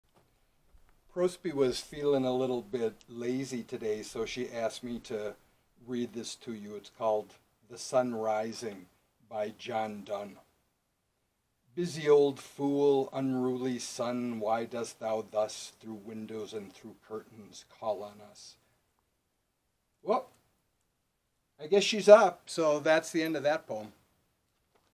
Prospi loves John Donne dogs johndunne poetry dogsandpoetry @poemoftheday
Assistant editor Prospi is a great fan of the Metaphysical poets, especially John Donne. She asked me to read this poem to you, "The Sun Rising." Find yourself a poetry collection or a short story or a novel or an essay and start reading?